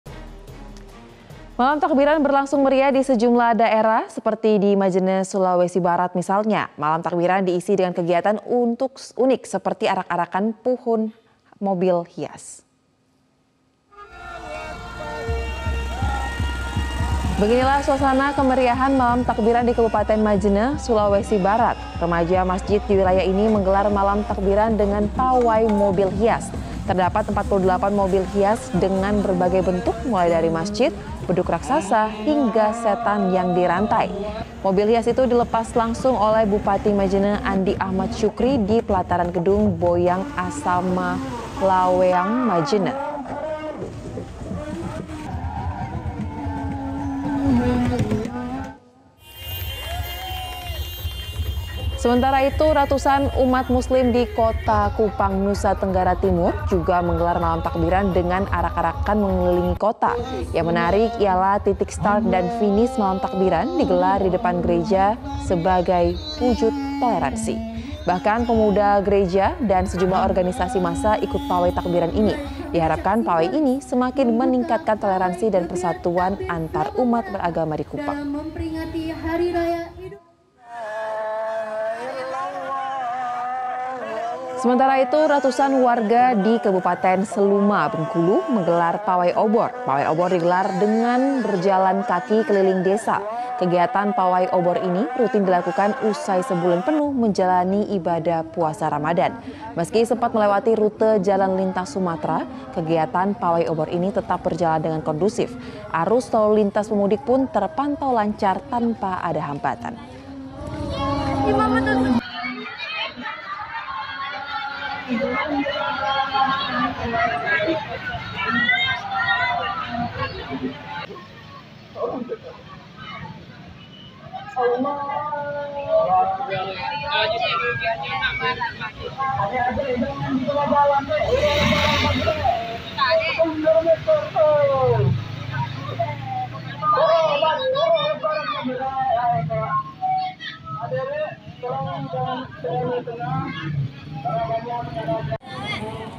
Kemeriahan Malam Takbiran di Sejumlah Daerah
MetroTV, Malam Takbiran berlangsung meriah di sejumlah daerah seperti di Sulawesi Barat.

Remaja masjid di wilayah Sulawesi Barat menggelar malam takbiran dengan pawai mobil hias, terdapat 48 mobil hias dengan berbagai bentuk.